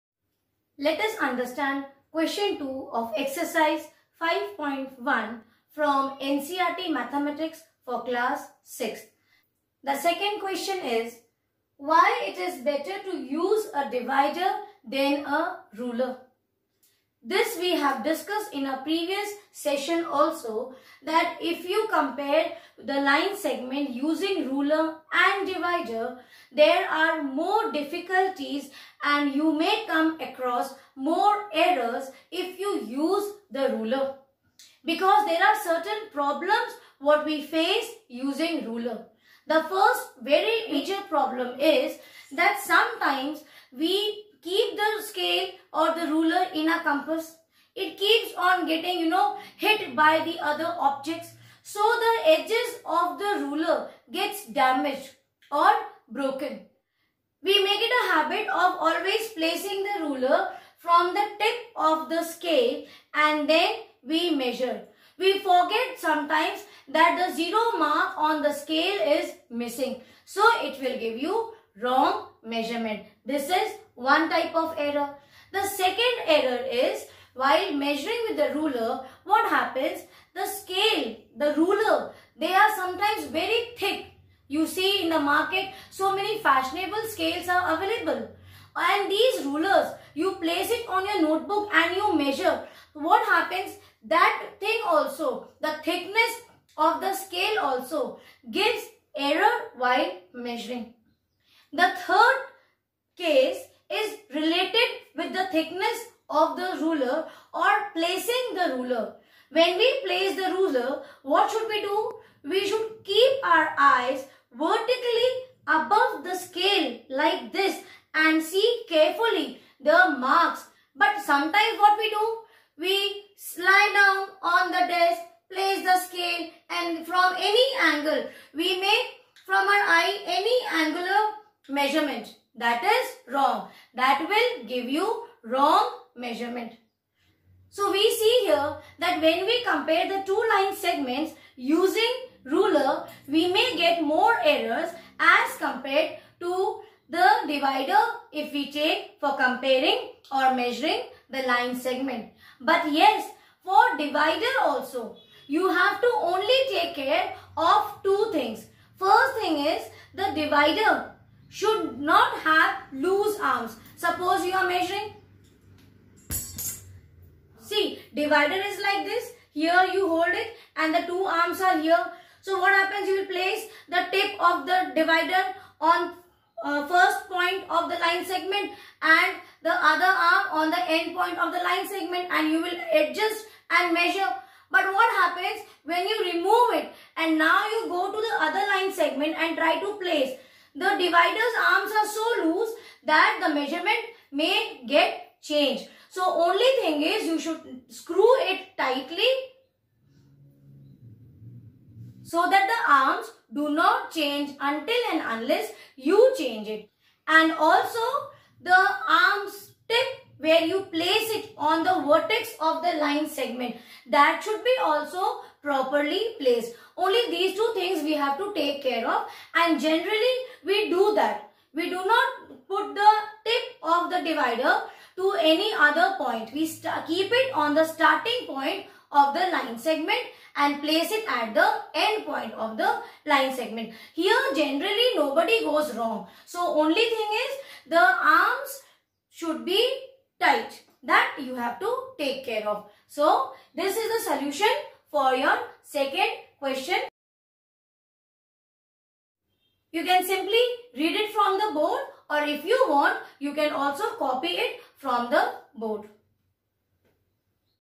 CLASS 6 | MATHEMATICS | CHAPTER 5 | EXERCISE 5.1 | QUESTION 2
In this video ,explanation is done on chapter 5,understanding elementary SHAPES, exercise 5.1,question 2 for class 6 ,ncert mathematics